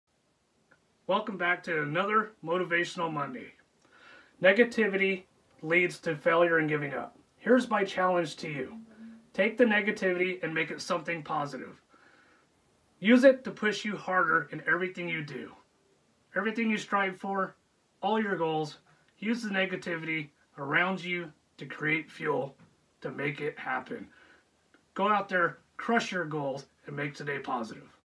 Motivation Monday!
Let’s take the Negativity around us and use it to fuel ourselves in everything we do! Turn Negativity into Positivity